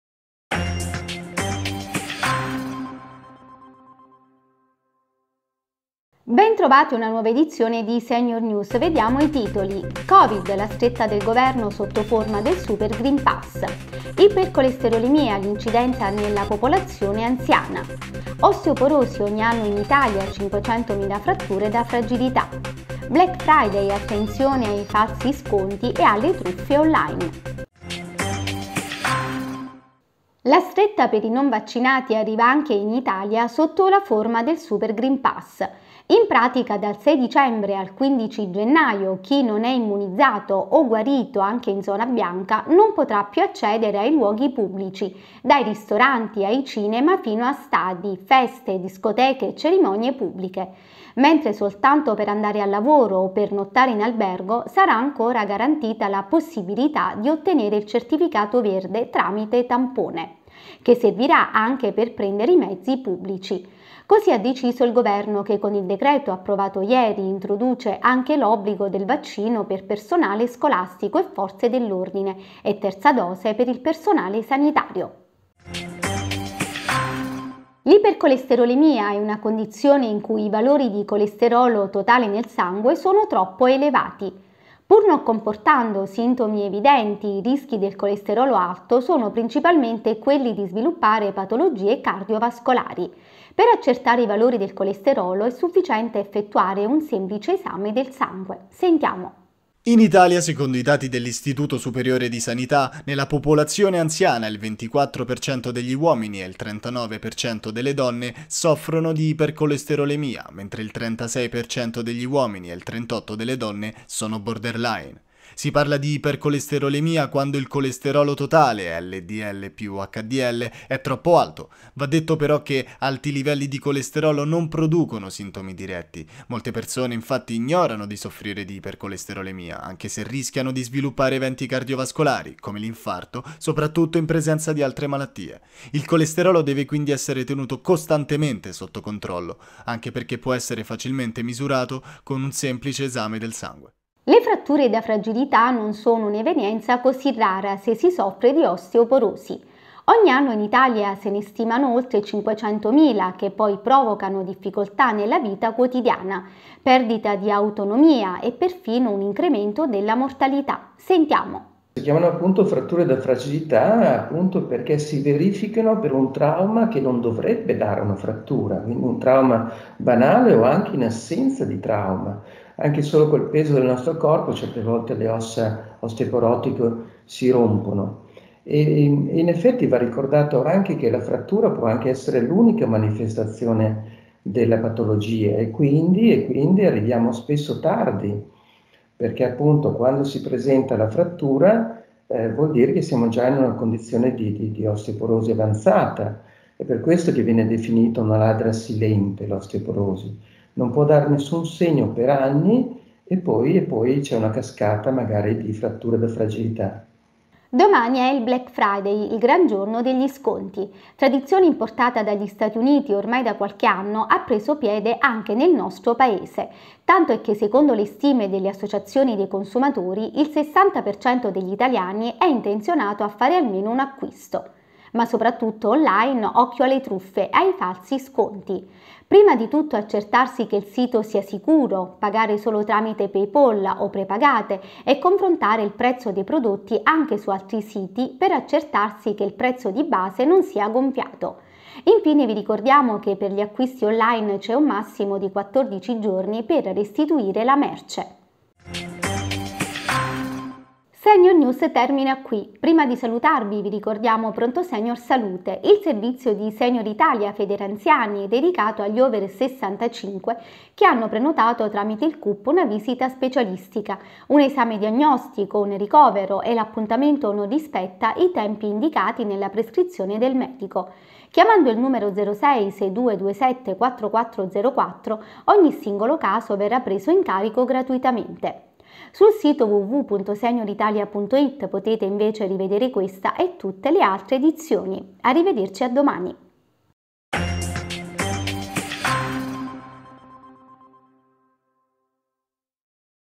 SENIOR NEWS 25/11/2021: NUOVE MISURE ANTI COVID/IPERCOLESTEROLEMIA/OSTEOPOROSI/BLACK FRIDAY
SENIOR NEWS 25/11/2021:
 - COVID, LA STRETTA DEL GOVERNO SOTTO FORMA DEL SUPER GREEN PASS
 - IPERCOLESTEROLEMIA, L'INCIDENZA NELLA POPOLAZIONE ANZIANA, SERVIZIO
 - OSTEOPOROSI, OGNI ANNO IN ITALIA 500 MILA FRATTURE DA FRAGILITA', INTERVISTA A: PROF. MAURIZIO ROSSINI - DIRETTORE UOC REUMATOLOGIA AOUI VERONA E PRESIDENTE SIOMMMS
 - BLACK FRIDAY, ATTENZIONE AI FALSI SCONTI E ALLE TRUFFE ON LINE